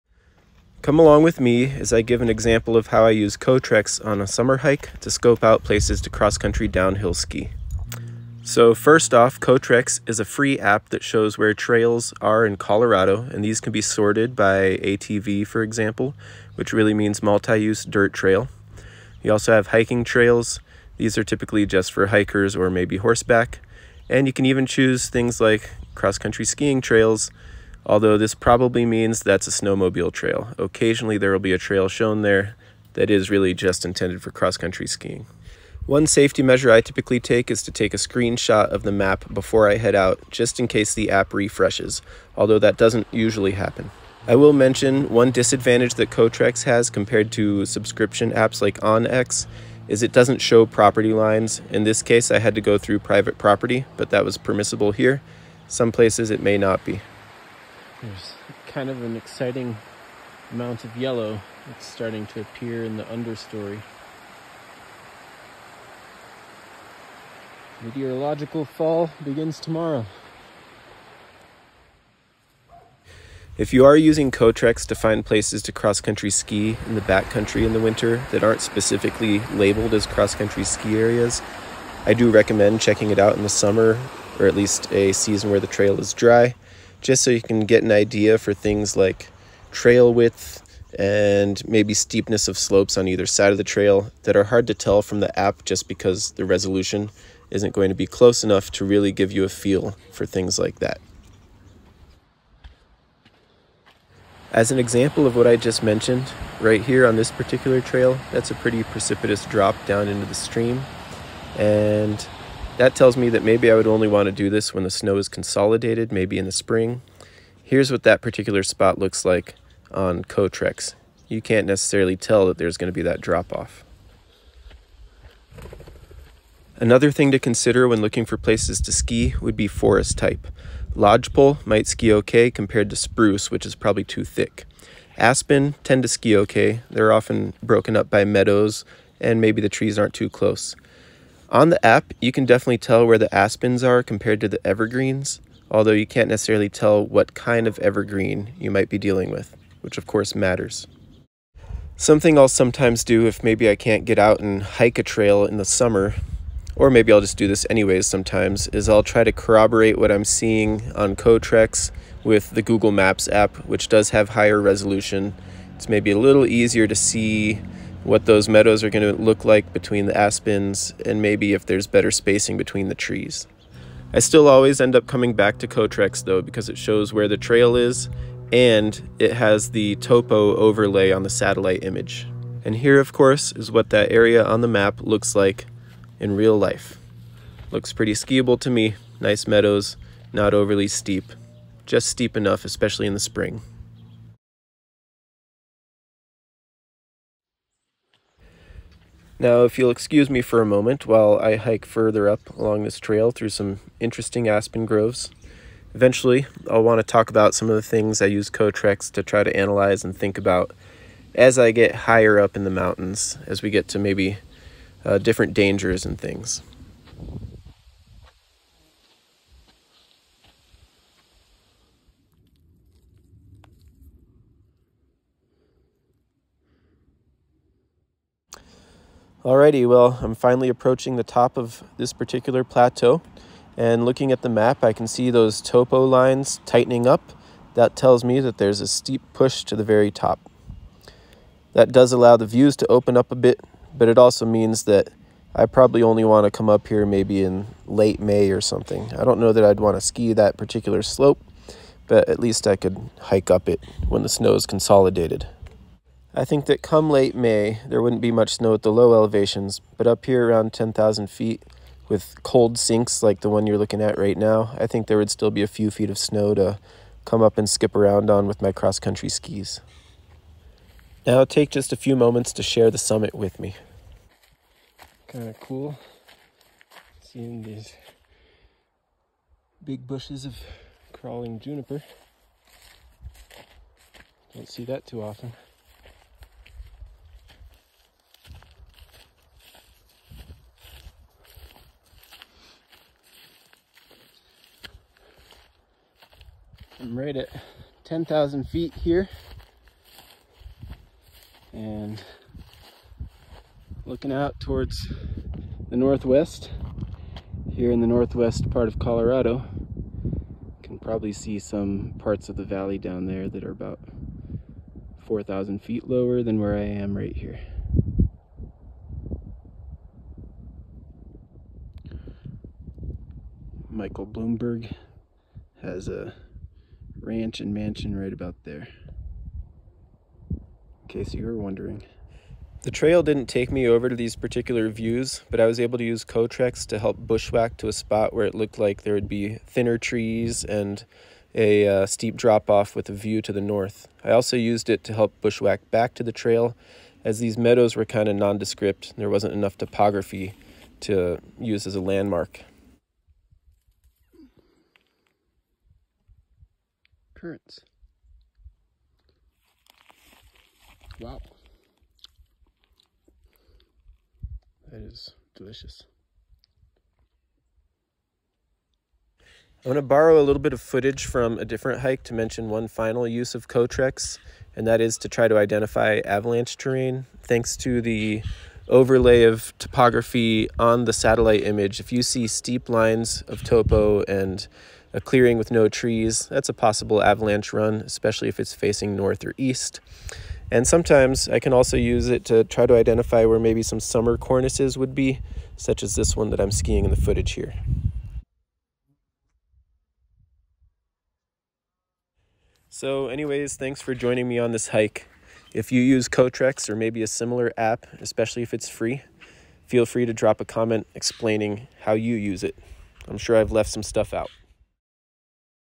Using COTREX to hike and scope out xcd ski spots
Whether you want to hike, ski, or do something else outdoors in CO, COTREX can be pretty handy. Here’s how I use it to hike and evaluate how suitable an area is for XCD skiing.